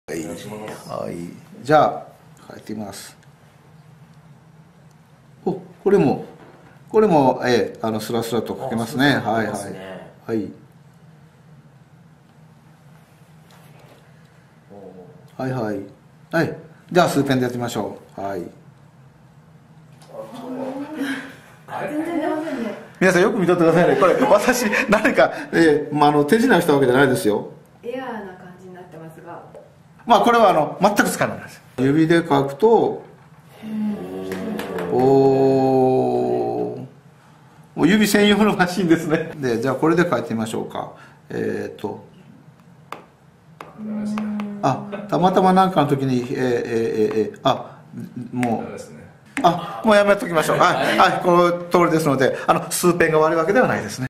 Andorid版7notesをナマでお試し（Optimus LTE L-01D）
MetaMoJi TV（2012/02/16）にて、Android版7notesを様々な機種で試しました。
手で書いた場合とスタイラスペン（Su-Pen）で書いた場合の両方の書き味を試しています。
その機種別の動画をアップします。
こちらは、Optimus LTE L-01Dです。